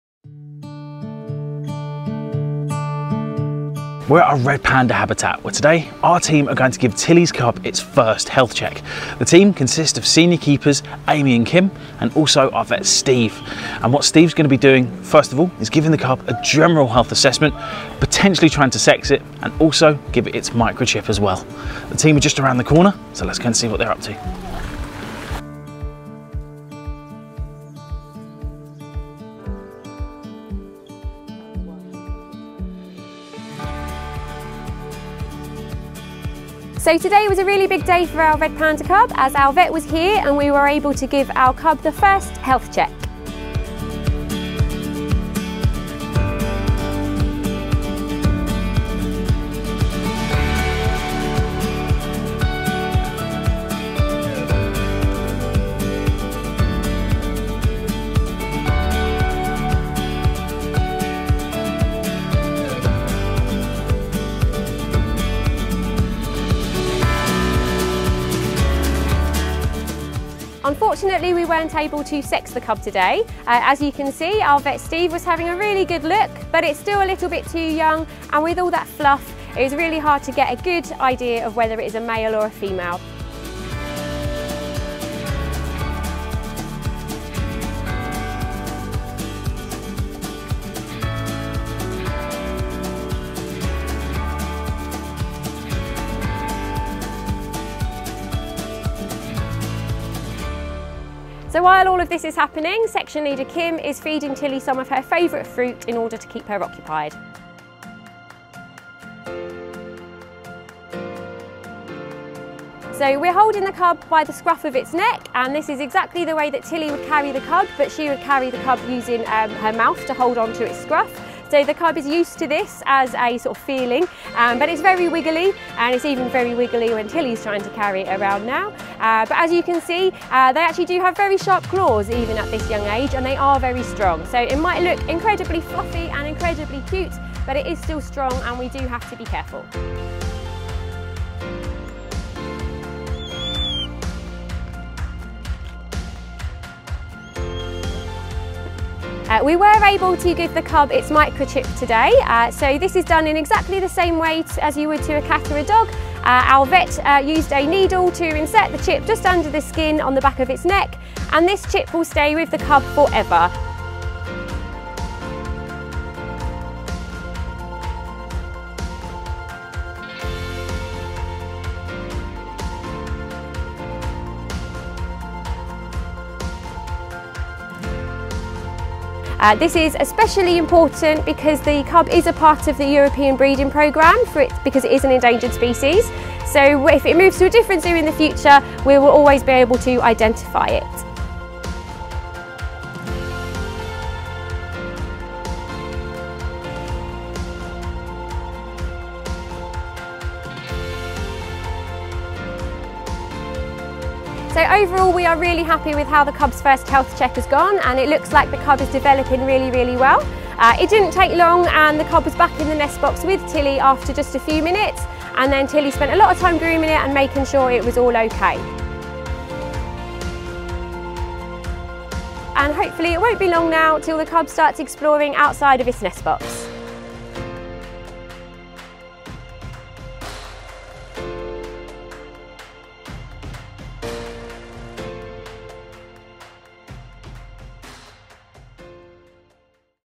Little Red Meets The Vet In First Health Check
Two months after the miracle birth of ‘Little Red’ the Endangered red panda at @HertfordshireZoo that stole the hearts of the world, it was time for the first health check with the vet.

In August, Paradise Wildlife Park announced the miracle birth of ‘Little Red’ after the tragic passing of their dad Nam Pang back in June. The story took the world by storm and Tilly the mum and ‘Little Red’ have become loved across the world for their heartfelt story.

Recently, as ‘Little Red’ has reached just over two months old, it was time for the vet to visit and to check how ‘Little Red’ is doing. The aim of the visit was to micro chip them and to see whether the sex can be told to hopefully find a name for ‘Little Red’.

Unfortunately, ‘Little Red’ was too much of a fidget and the vet was unable to tell the sex of them. Their cheeky personality is definitely coming through and this will have to wait until a later date. ‘Little Red’ was able to have their microchip fitted and this will help identify them and follow them as they grow older.

After the visit, mum Tilly and ‘Little Red’ were reunited in their cosy nest box and were seen through the CCTV cameras having a cuddle and a little nap.

Red Pandas are classed as Endangered by the International Union for Conservation of Nature (IUCN). Human exploitation of forest resources and fragmentation has caused a 50% decline of the species in the last 20 years, leaving approximately 2,500 remaining in the wild. Tilly and ‘Little Red’ continue to act as ambassadors for their wild counterparts, helping to educate the public and raising awareness for the species.

Paradise Wildlife Park will continue to share exciting updates of Tilly and ‘Little Red’ across their social media channels and website with the hope soon there may be tiny paws in the big outdoors!